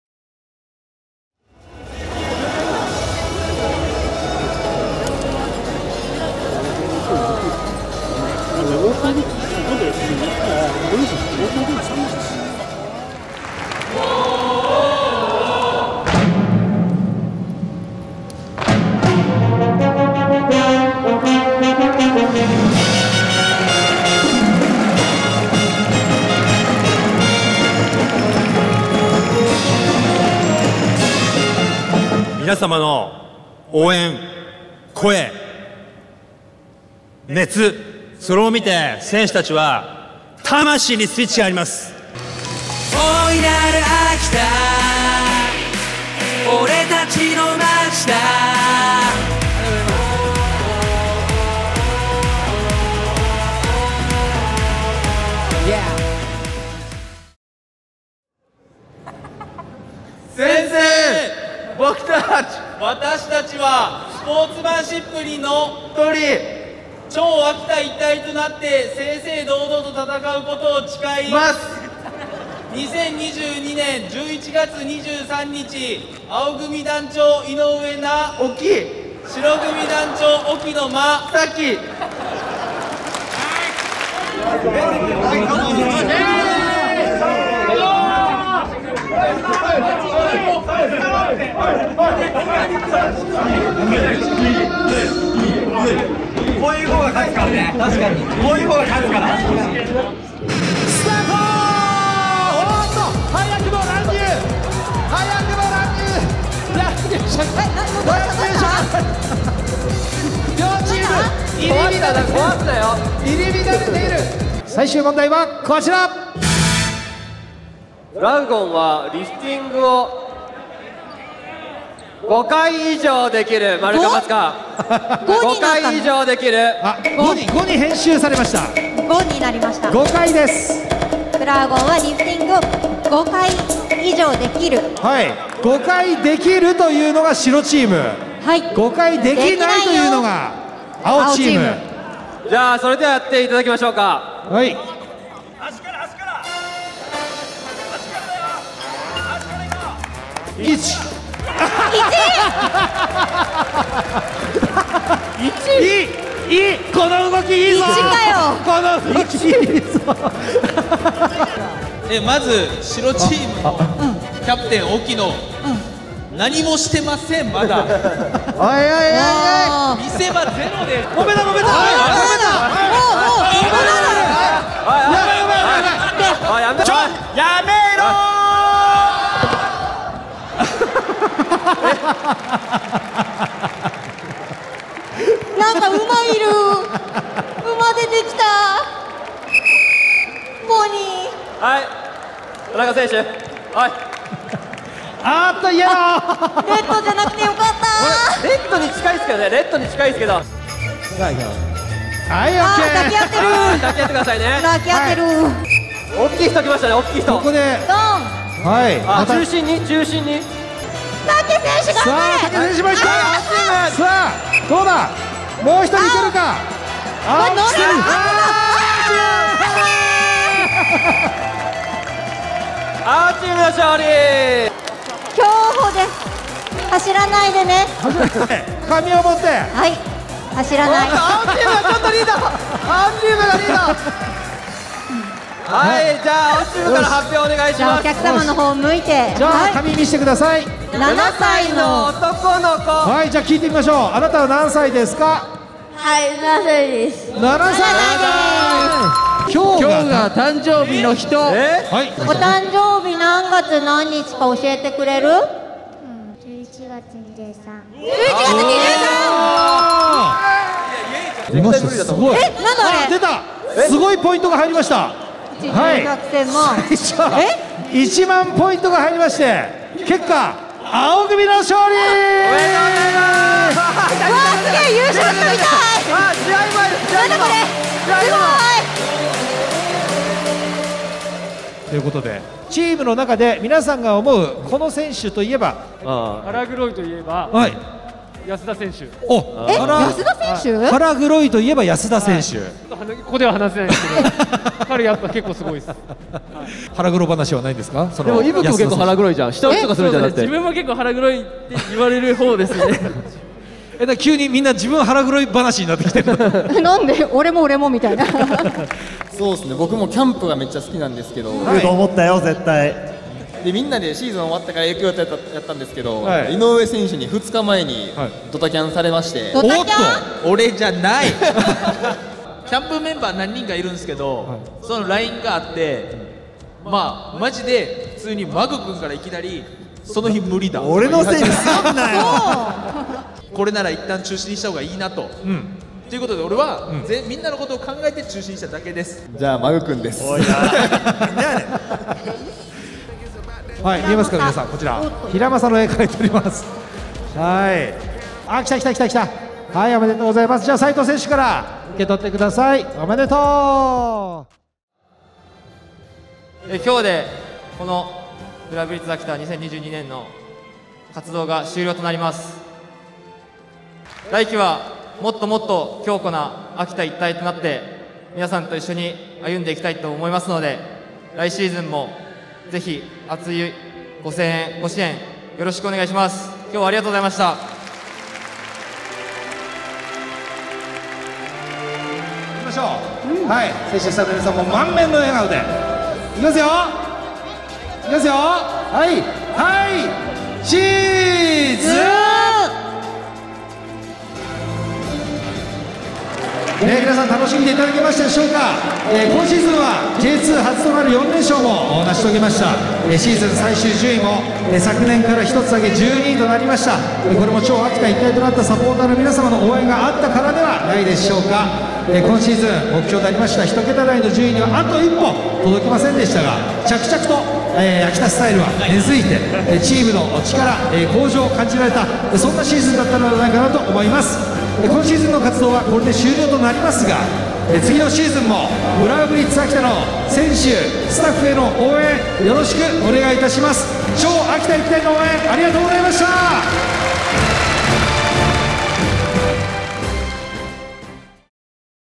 【ダイジェスト】ブラウブリッツ秋田ファン感謝祭2022
11月23日、由利本荘市のナイスアリーナで開催した「ブラウブリッツ秋田ファン感謝祭2022」のダイジェスト動画をお届けします。
当日は、羅漢ライブ、大運動会、トークショーなどが開催されました。